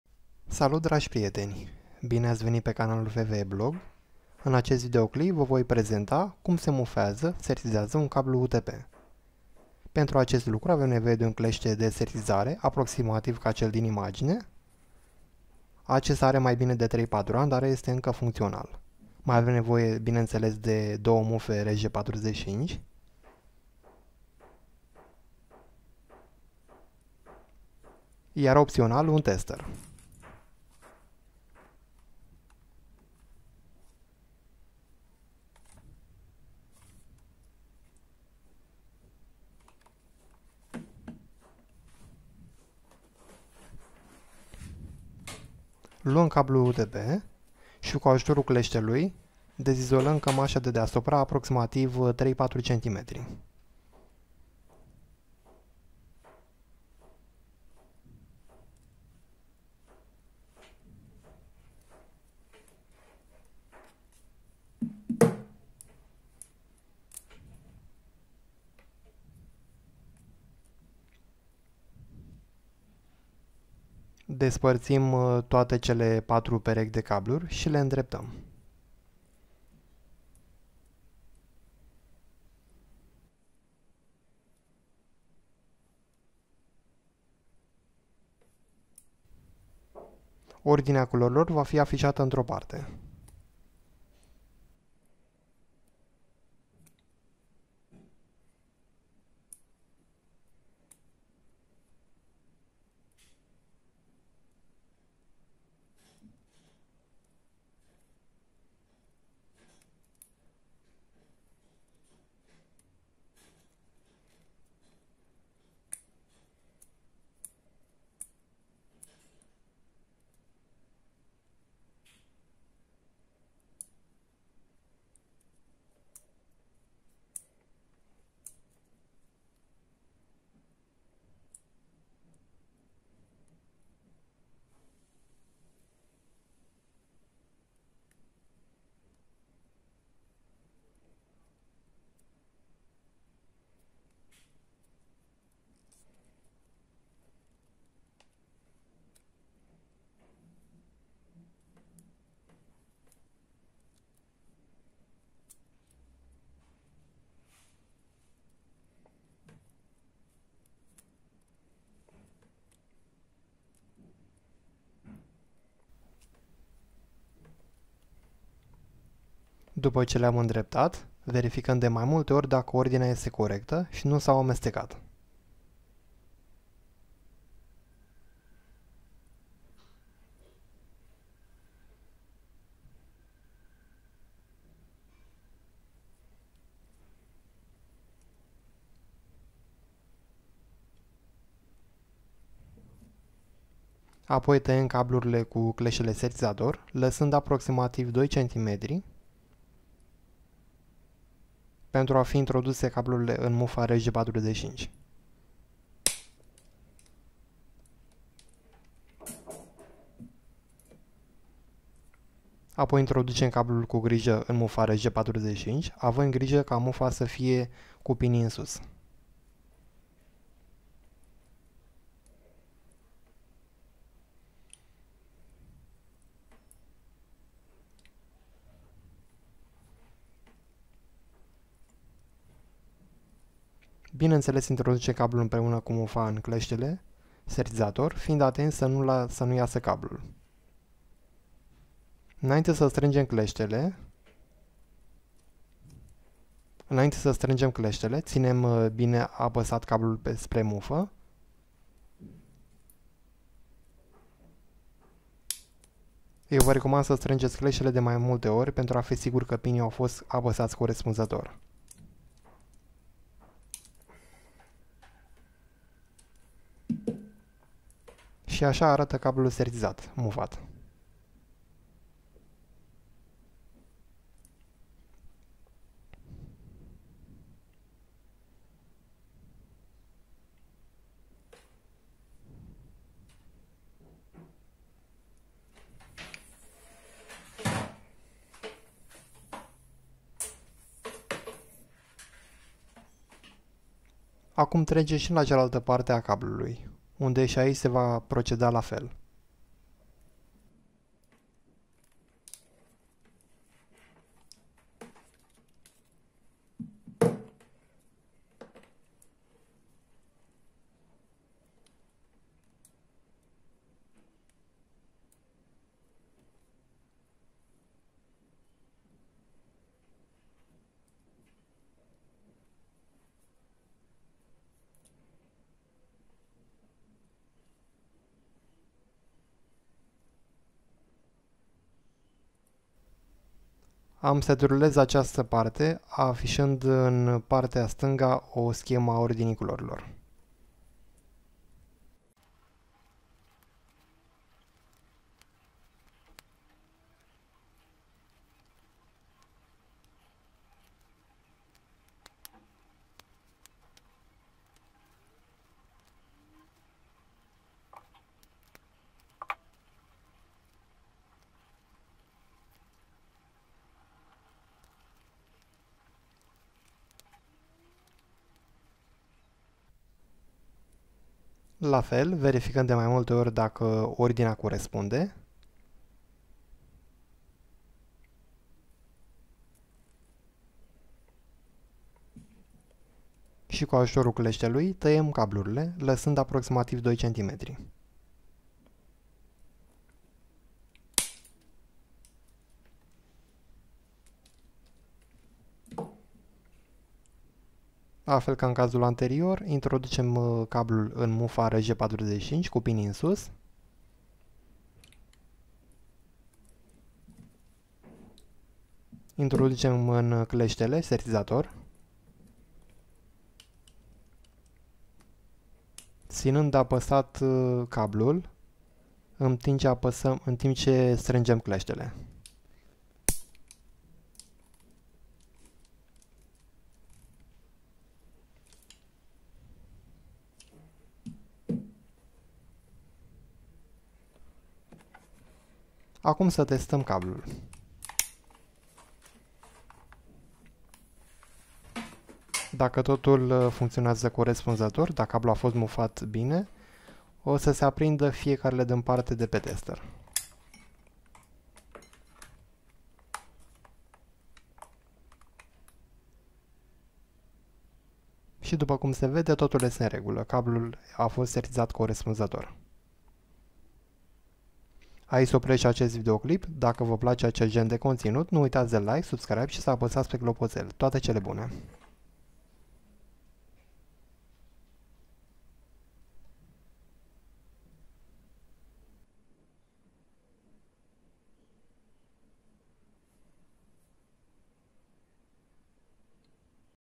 Cum se mufeaza/sertizeaza un cablu de internet
In videoclipul prezent am explicat si aratat cum se mufeaza(sertizeaza) un cablu UTP(de internet). Denumit si cat5e, cat6.
     Nu uitati ca aceasta este metoda mea  si poate sa nu corespunda cu modul altor. Dar pentru cablu de internet aceasta este ordinea corecta a culorilor.